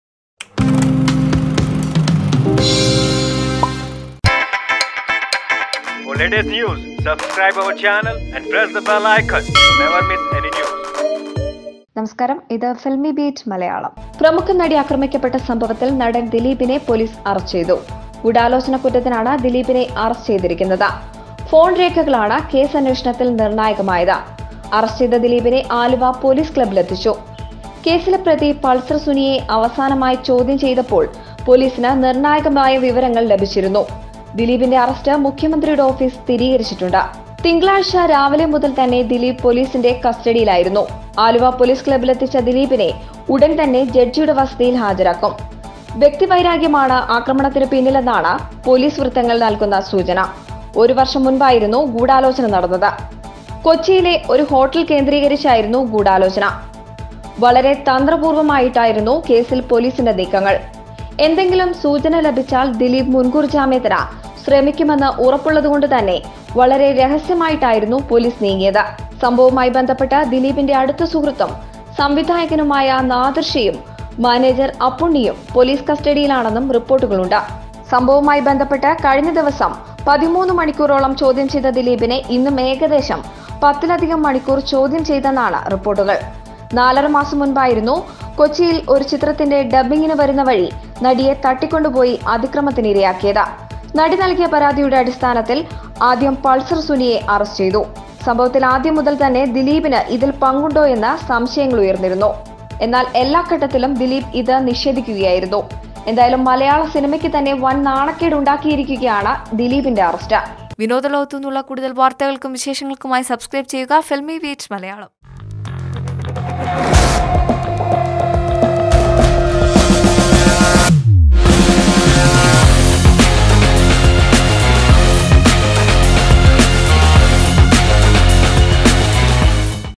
Dileep Arrested In Actress Abduction Case | Filmibeat Malayalam
After rounds of questioning, actor Dileep was arrested today in connection with the actress abduction case.